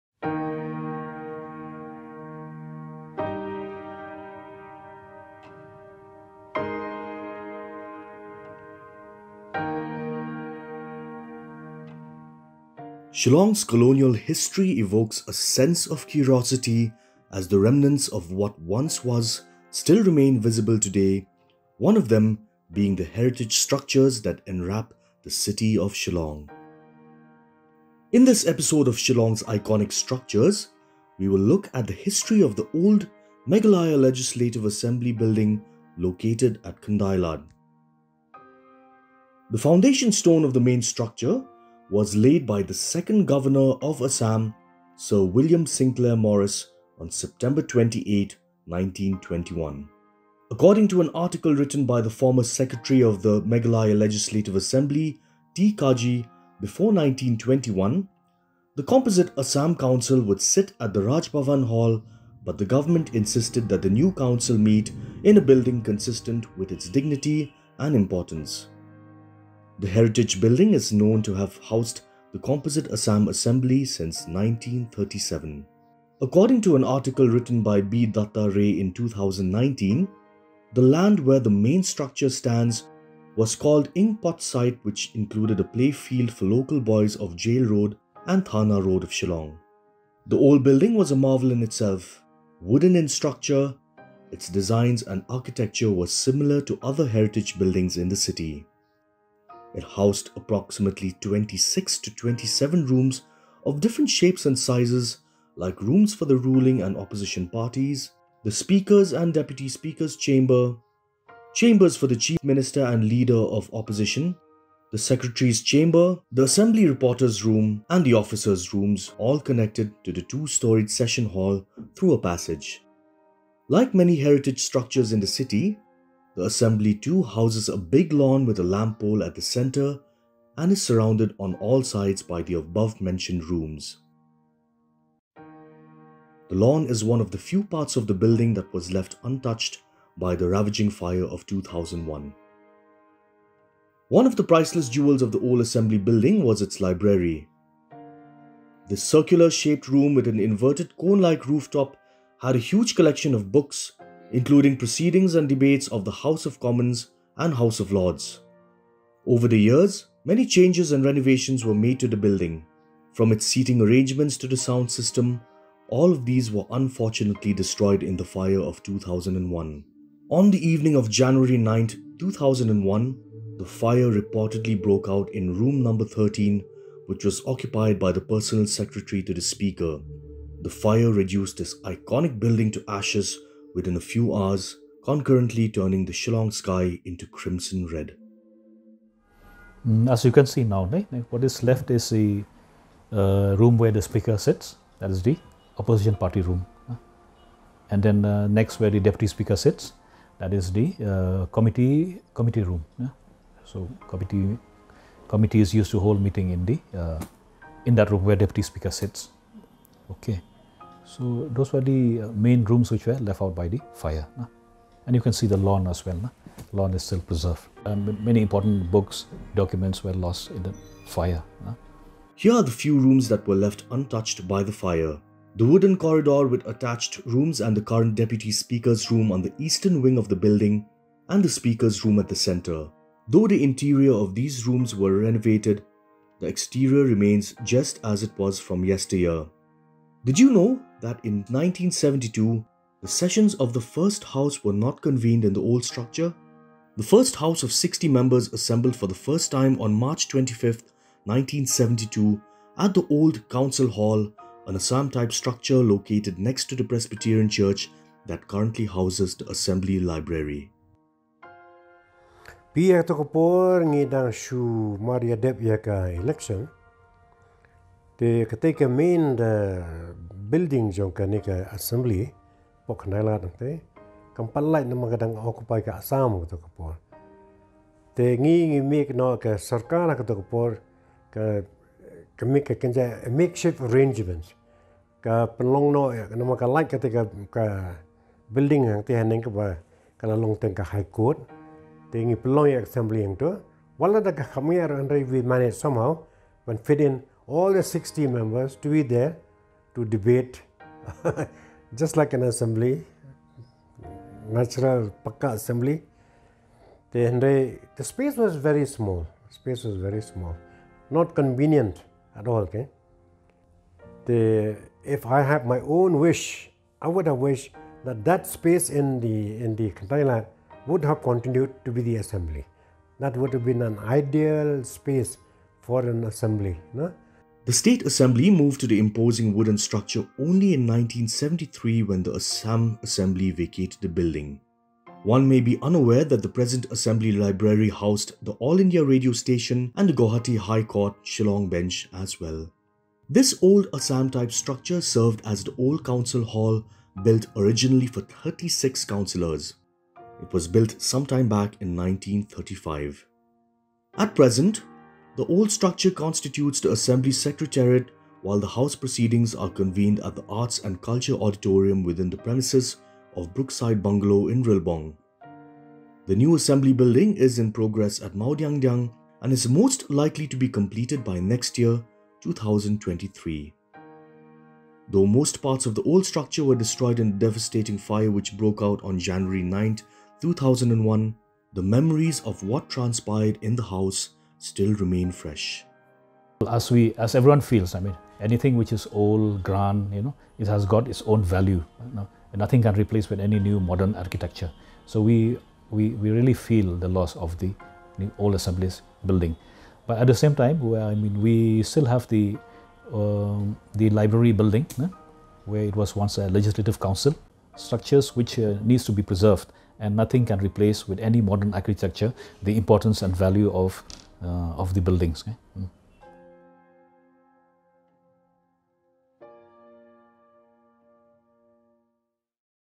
Old Assembly Building: A stately structure before being engulfed in flames
Shillong’s colonial history evokes a sense of curiosity as the remnants of what once was, still remain visible today - one of them being the heritage structures that enwrap the city of Shillong. In this episode of Shillong's Iconic Structures, we will look at the history of the old Meghalaya Legislative Assembly building located at Khyndai Lad.